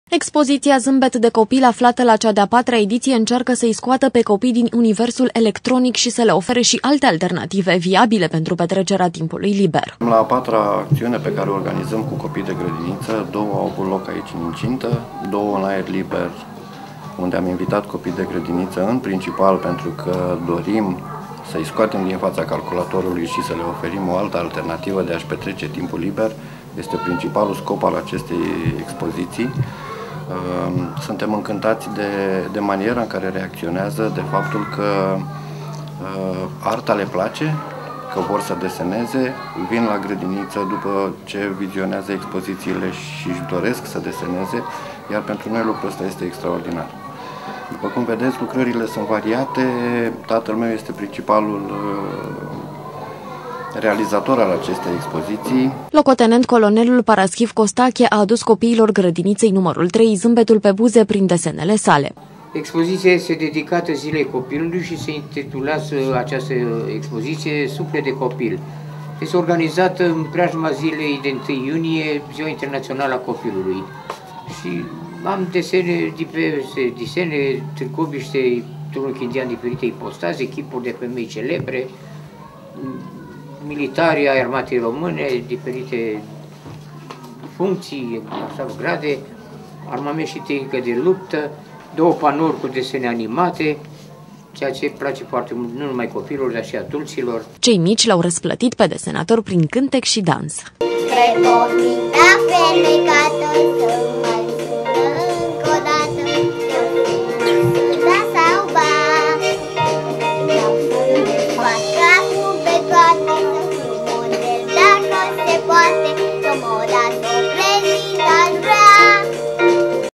ZÂMBET DE COPIL (Columna Tv)
Pentru ca se apropie ziua internationala a copilului, gradinita nr 3 din Targoviste a gazduit o expozitie de desen realizata de locotenet-colonelul in retragere Costache Paraschiv Manole Teodor.Expozitia Zambet de copil, aflata la cea de-a patra editie incearca sa-i scoata pe copii din universul electronic si sa le ofere si alte alternative viabile pentru petrecerea timpului liber.